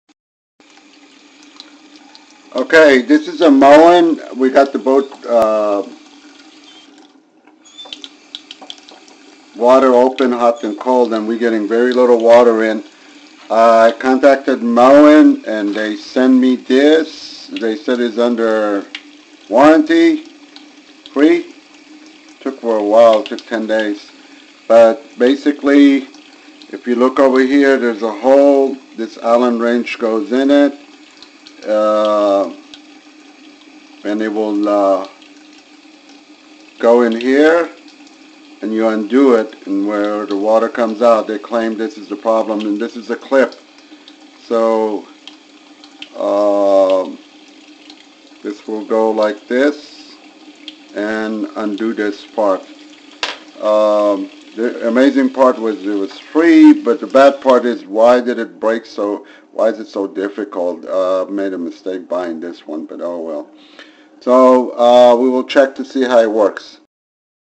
Moen oxby clogged
fix the low flow problem for free on a Moen  oxby  in 10 min